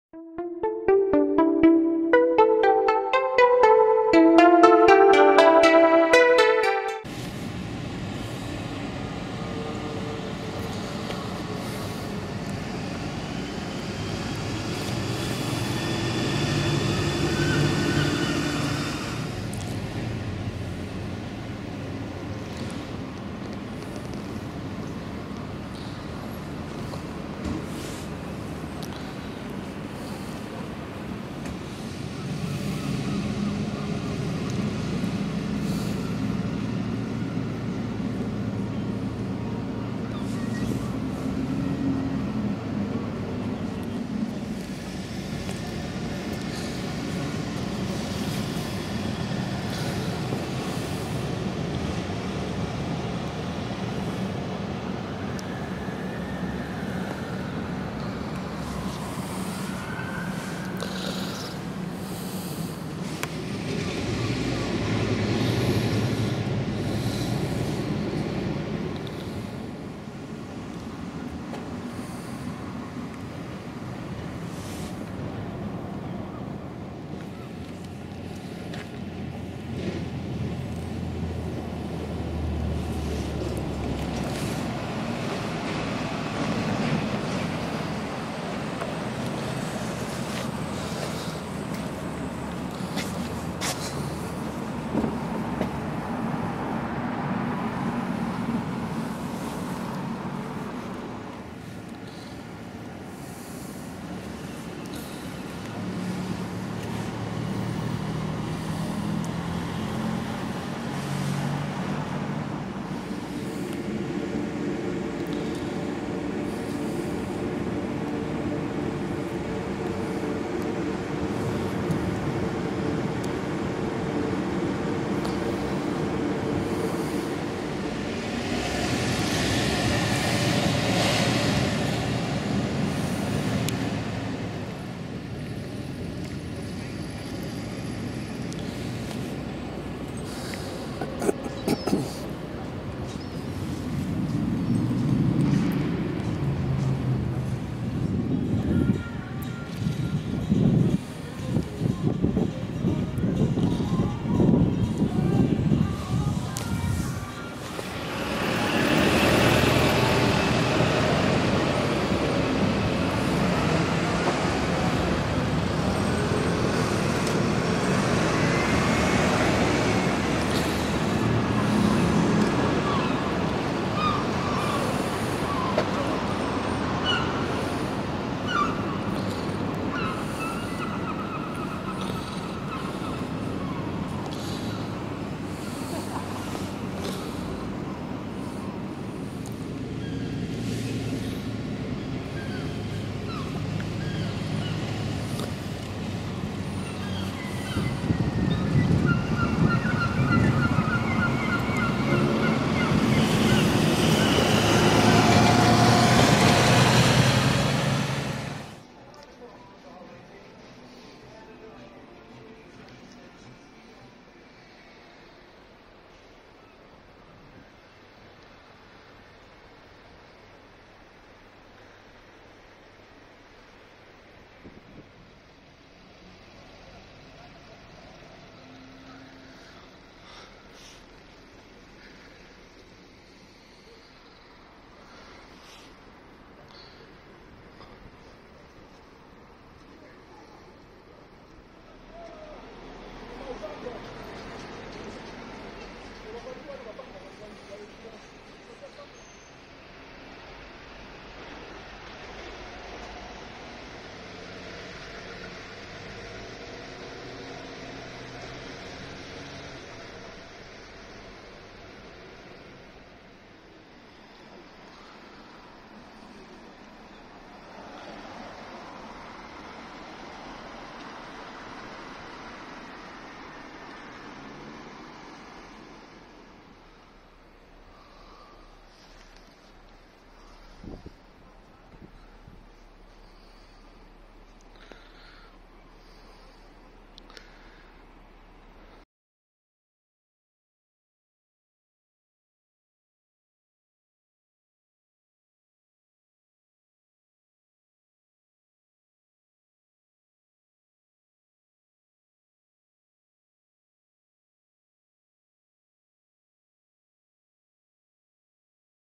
Lime Street Redevelopment Part 1
My first visit over to Lime St, Liverpool to get some footage of the major re-development currently being worked on.  The area is a major route for cars and buses, and is one of the first things people see when getting of the train at Lime St Station.  a multi million development is in progress.  I will go over again over the next few months.